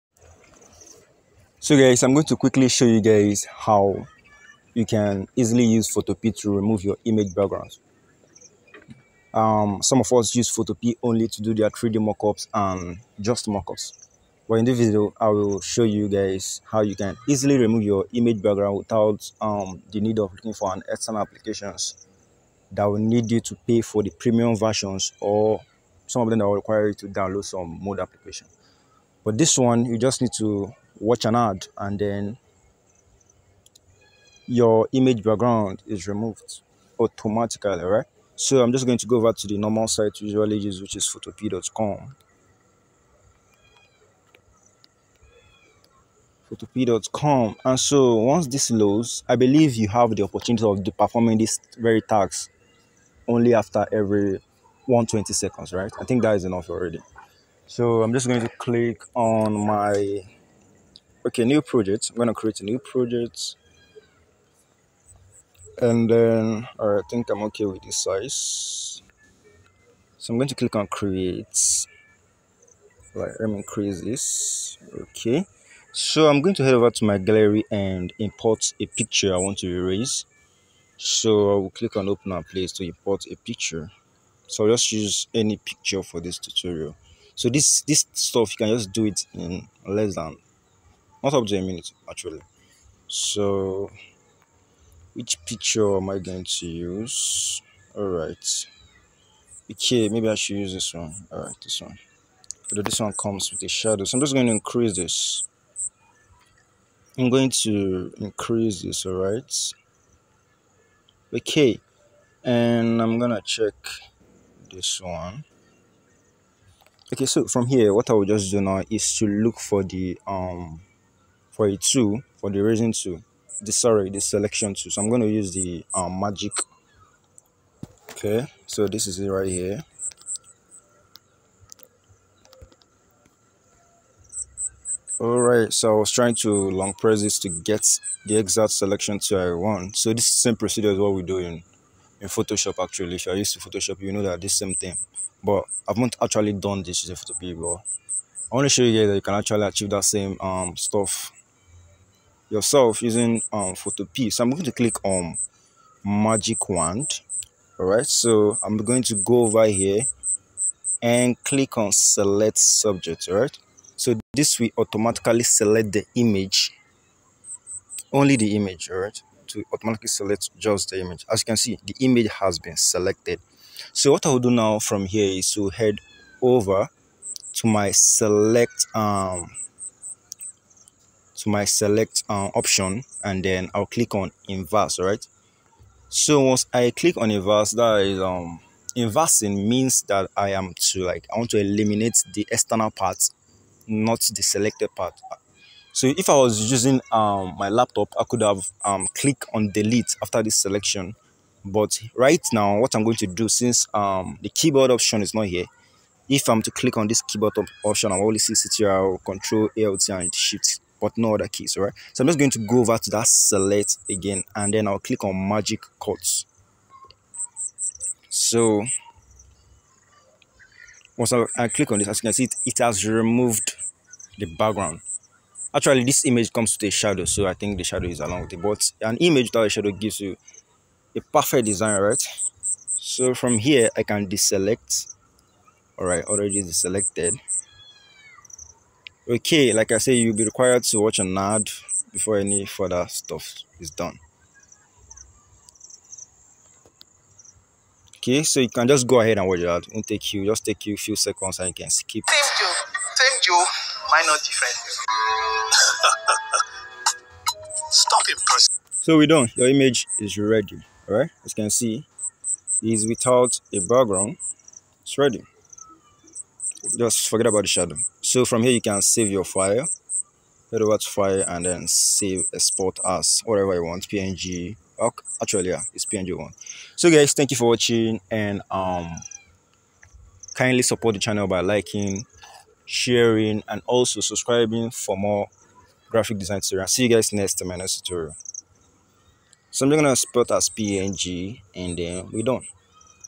How to Quickly Remove Image Background using Photopea on your Smartphone just like Photoshop
This YouTube video demonstrates a quick and easy method for removing image backgrounds using the Photopea app on your smartphone just like Photoshop. It's a handy guide for anyone looking to create stunning images with transparent backgrounds without the need for complex software.

NEED A PROFESSIONAL💎 DESIGN FOR YOUR BUSINESS🎓 OR BRAND🎓

Usefull links
pixellab 🎤🎧